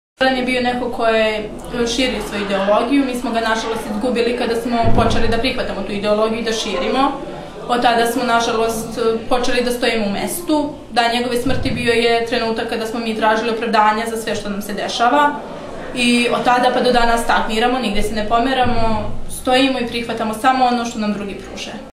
Nevena Ilić, Sirogojno
Šta je za mene značio Zoran Đinđić - Nevena Ilić, Sirogojno